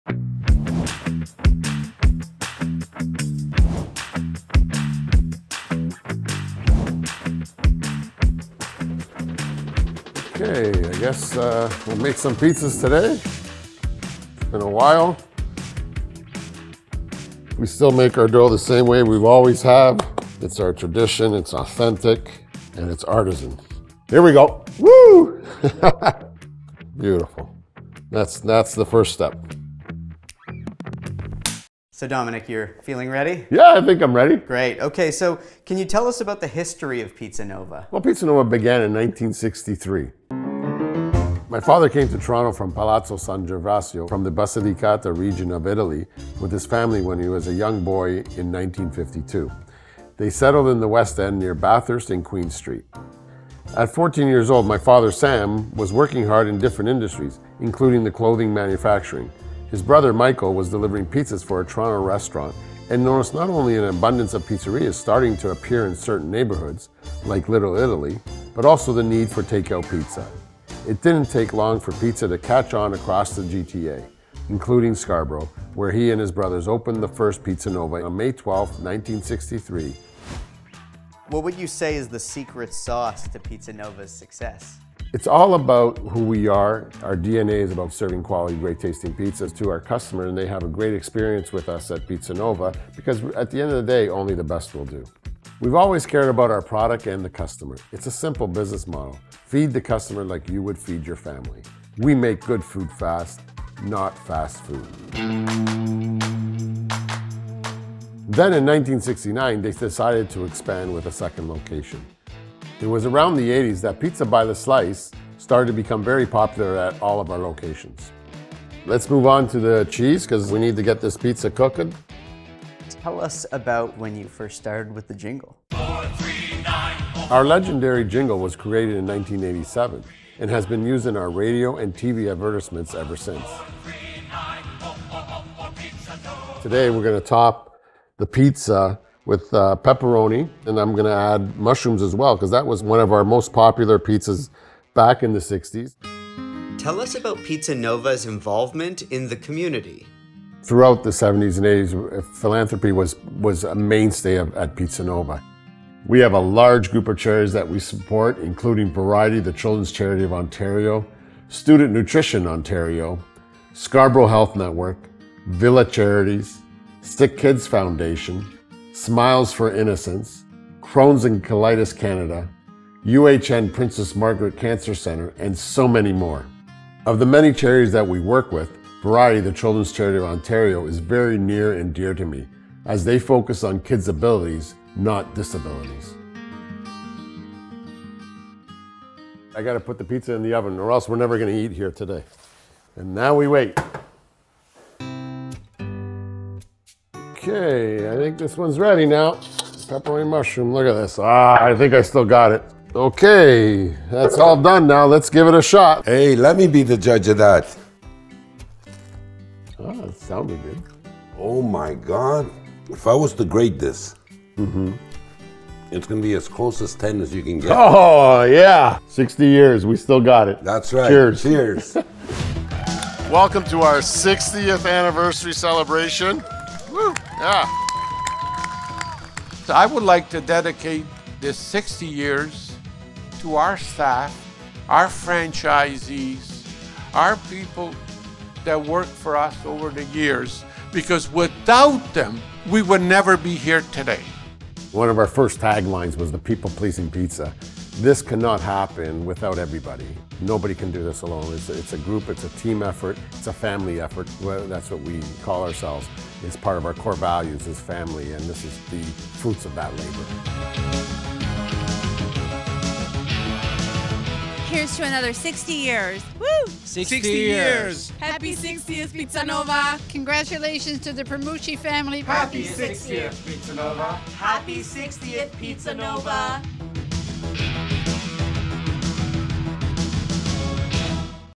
Presidential Pizza: Our Commander-in-Chef Shows Us How It's Done!
Making authentic and delicious pizza is a part of our DNA.

In celebration of our 60th Anniversary, we sat down with our President, Domenic Primucci, to see if he still has the chops to make his family recipe.

What's his secret? Feed the customer like you would feed your family.

He also had plenty to say about our involvement in the community, our iconic jingle, our history, and more.